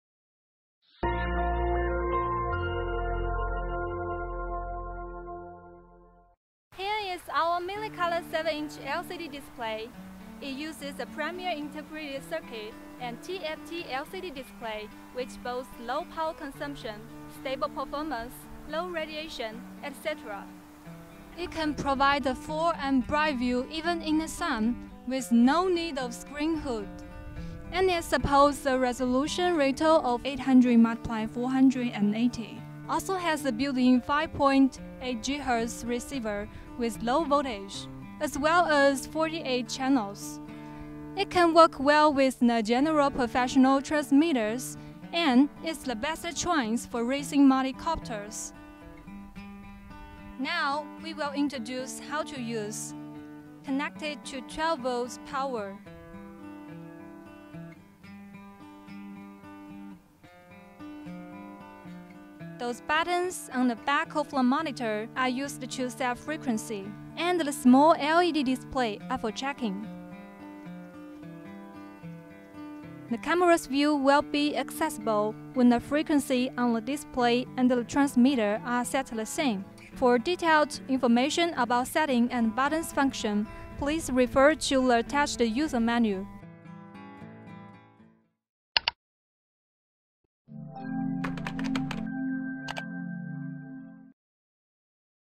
SunFounder Mini Color 7-inch 800*480 FPV TFT LCD Monitor for Racing Drone Multicopter
This SunFounder Mini Color 7-inch LCD Monitor provides great convenience on aircraft FPV flying. It’s easy to use with its small size and integrated circuit. In addition, it can provide a full and bright view even in the sun. It is really a best choice for racing multicopters.
If you have any technical problems, post forums on our